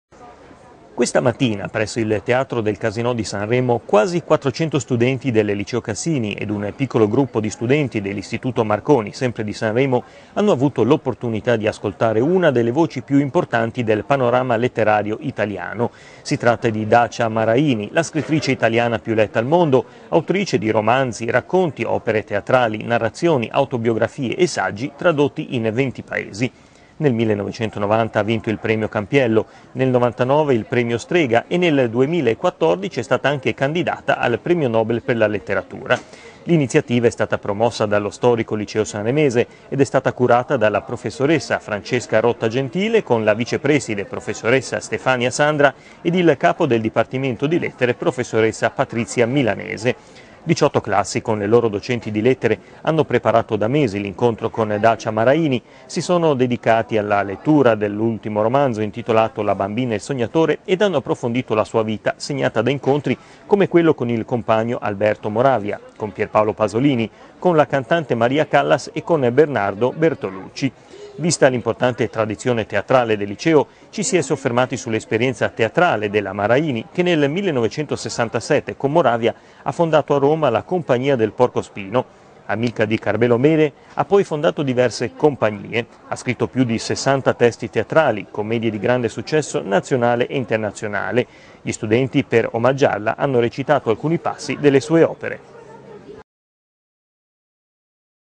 INCONTRO CON DACIA MARAINI
Quasi 400 studenti nel teatro del casinò di Sanremo hanno avuto l’opportunità di incontrare Dacia Maraini. Si tratta della scrittrice italiana più letta al mondo, autrice di romanzi, racconti, opere teatrali, narrazioni, autobiografie e saggi.